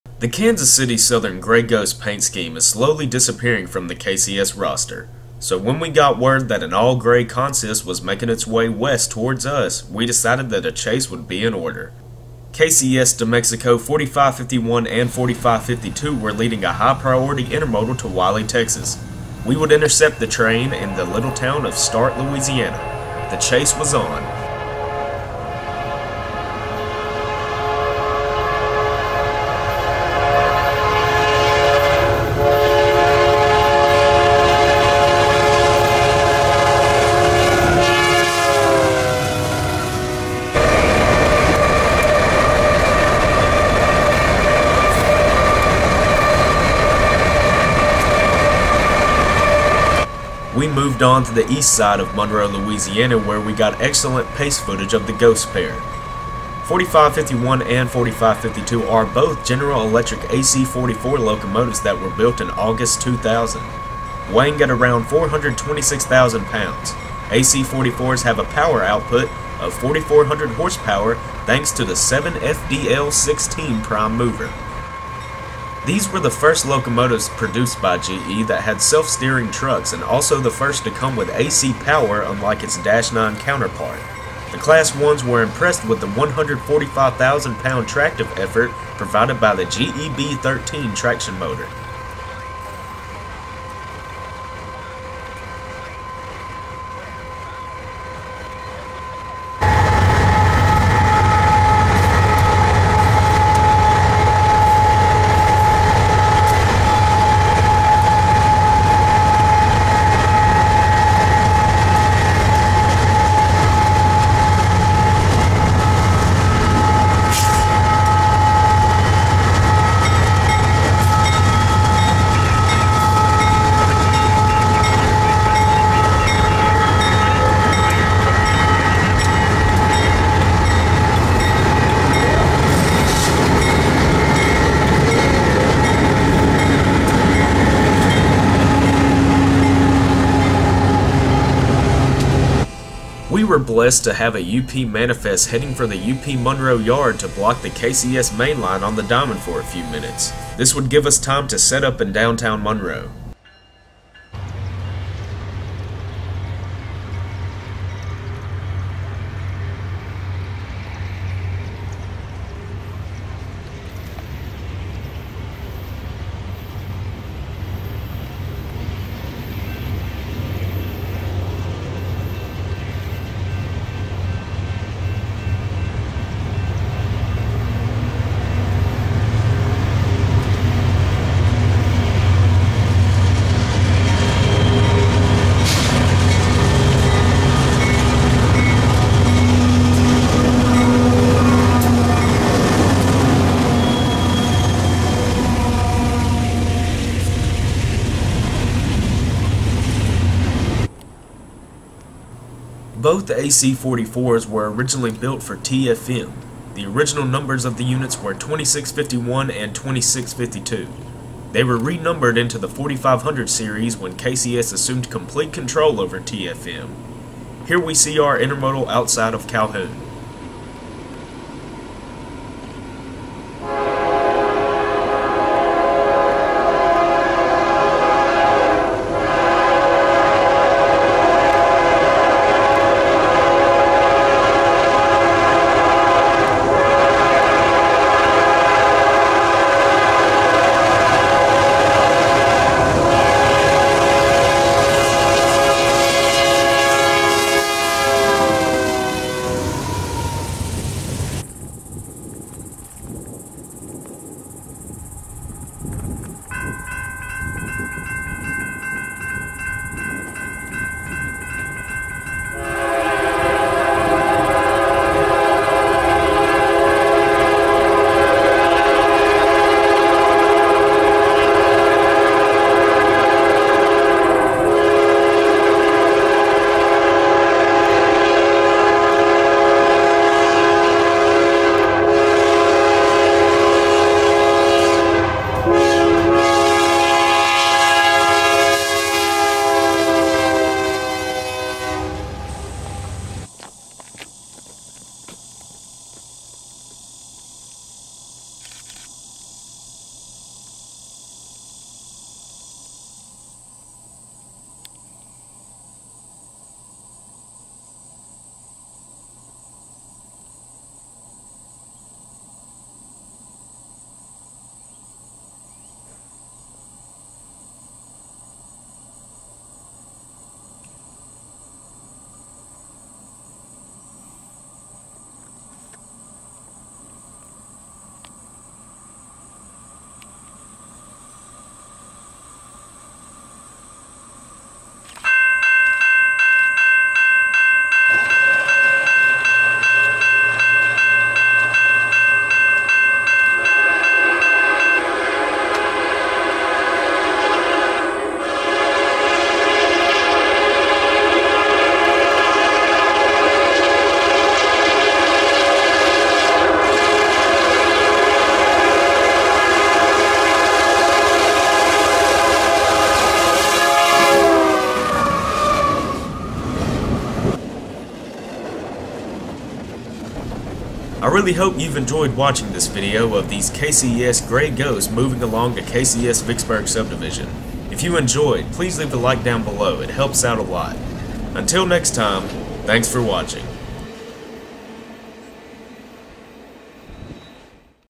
Chasing Kansas City Southern Grey Ghost AC44's!
Not as common as it was in 2006 to see all grey consists. Enjoy!